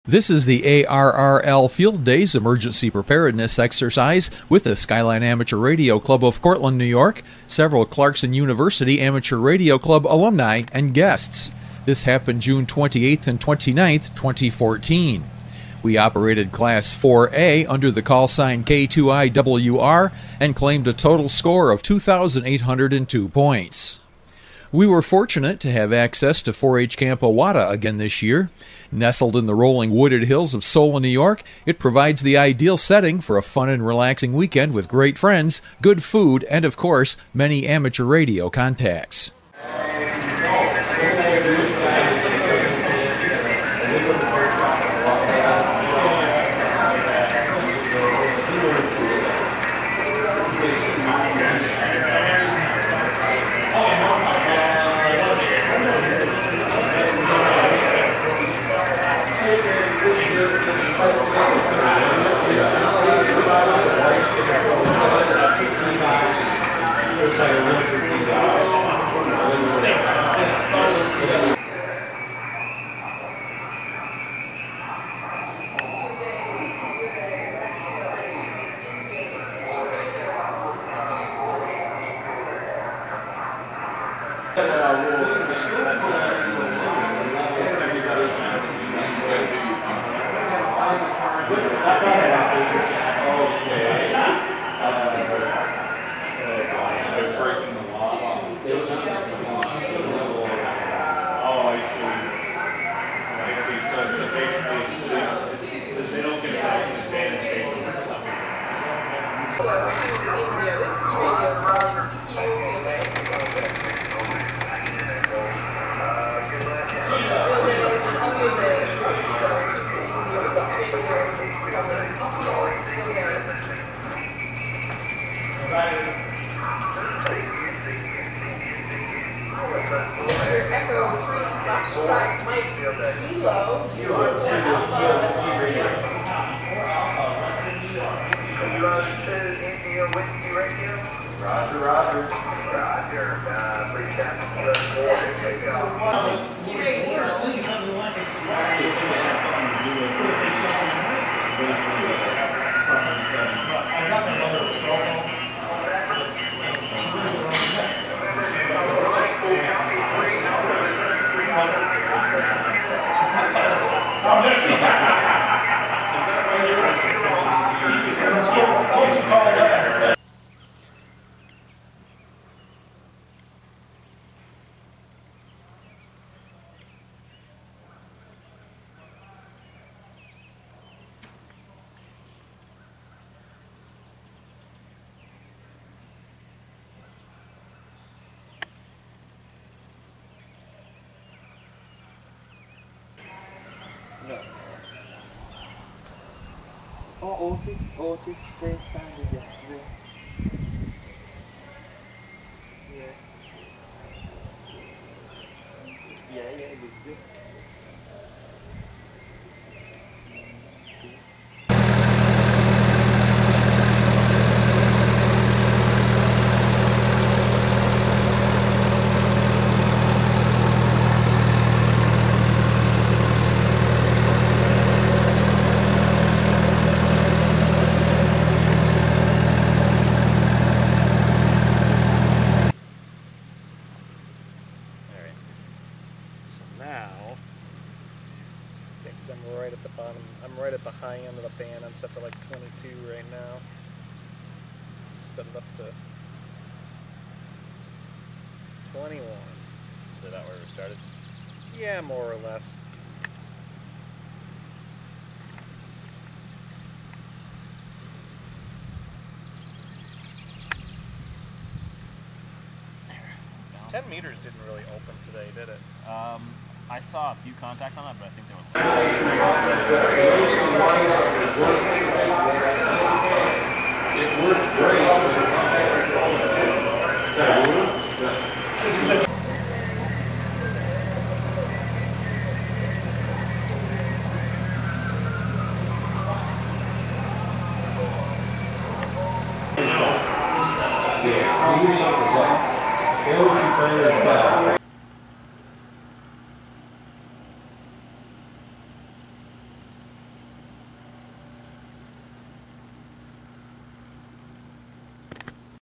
Field Days 2014
Skyline Amateur Radio Club field day at 4-H Camp Owahta in Solon NY.